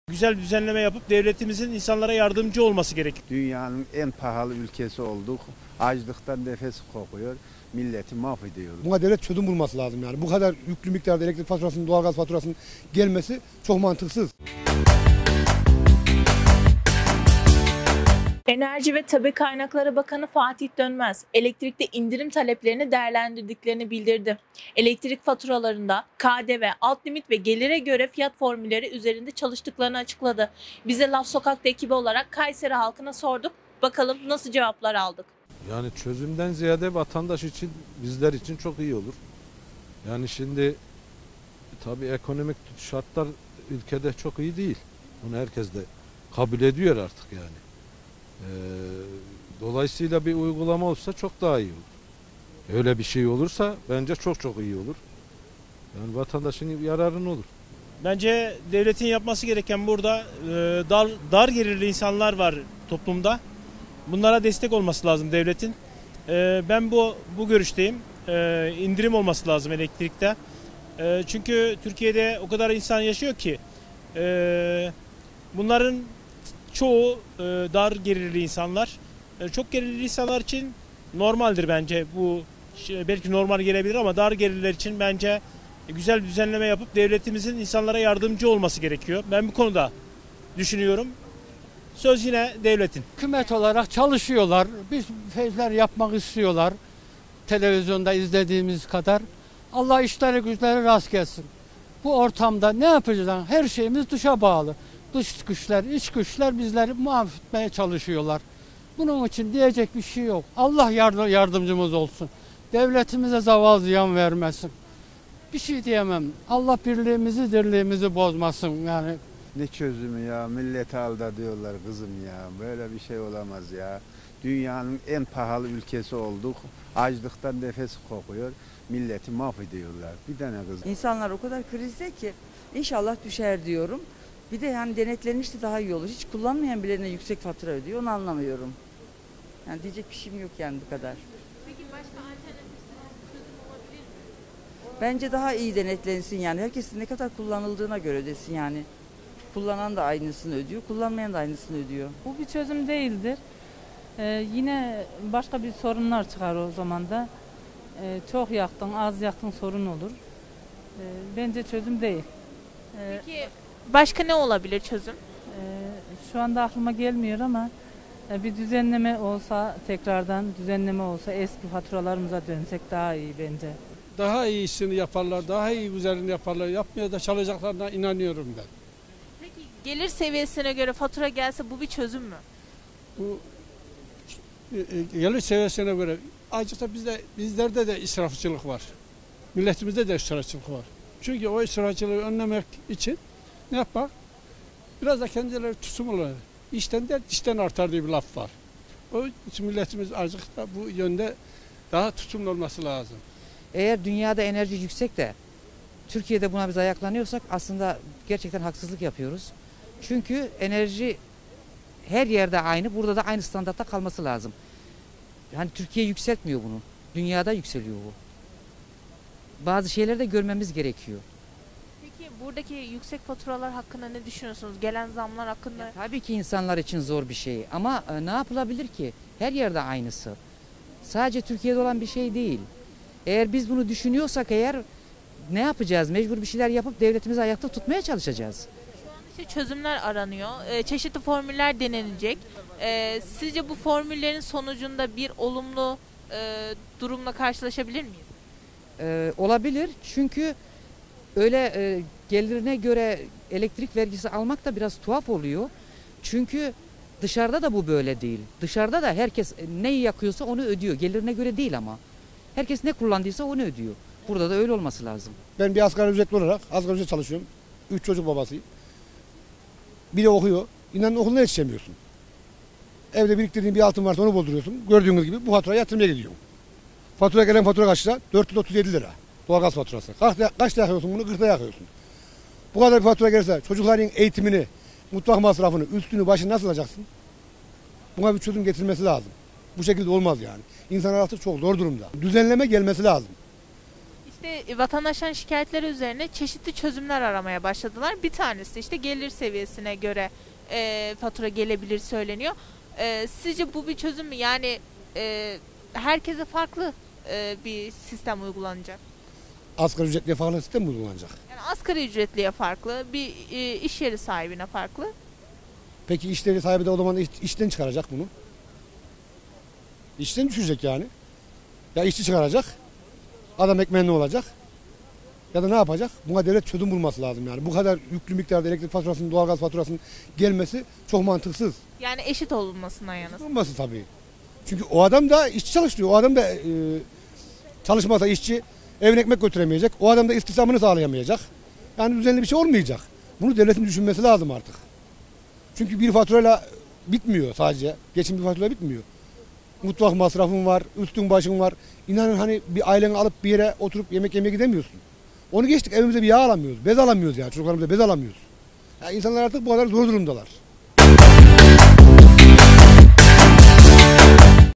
⭕️🎙Elektrik Faturalarında Gelir Düzenlemesi
Enerji ve Tabii Kaynaklar Bakanı Fatih Dönmez, çeşitli sosyal kesimlerden indirimli elektrik tarifesinden yararlanmaya dönük taleplerin olduğunu belirterek, "Hem Bakanlığımız hem de Enerji Piyasası Düzenleme Kurumu (EPDK) bu talepleri değerlendiriyor" ifadelerini kullanmıştı. Elektrik faturaları zamlar sonrasında yeniden düzenlenirken Kayserim.net Laf Sokakta ekibi olarak vatandaşlara "Elektrik faturalarında ki düzenlemeleri sorduk. Peki siz ne düşünüyorsunuz?